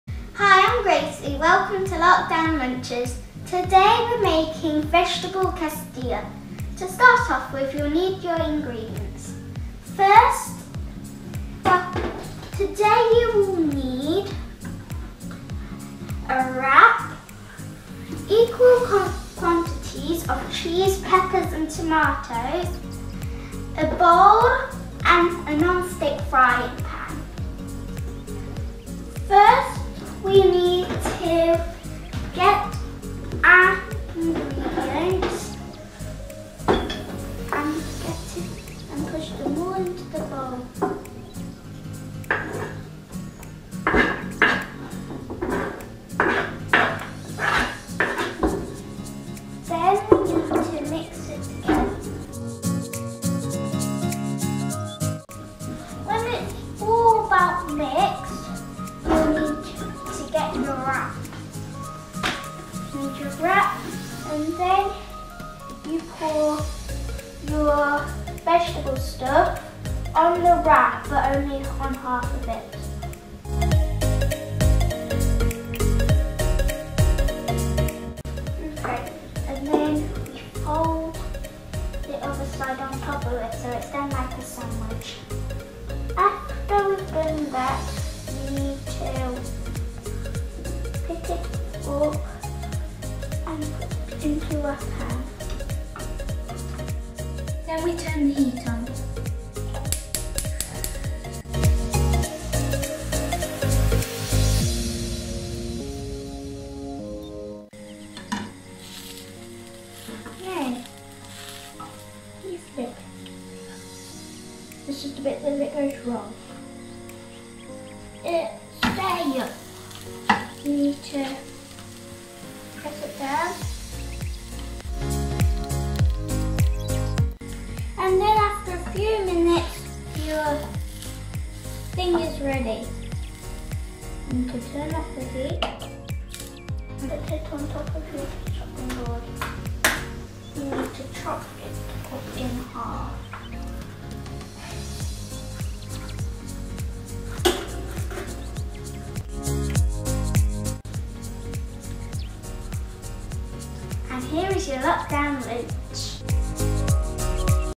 Gracie V's Lockdown Lunch Ideas - Veggie Quesadilla - Short version
Lockdown Lunches is about showing how simple lunch ideas for kids can be made. Gracie is a 9 year old who loves cooking, and wants to share her lunch recipes with others.

Veggie Quesadillia

Ingredients

1 Tortilla Wrap
1 Salad Tomato
50g Cheddar Cheese
1/2 Coloured Pepper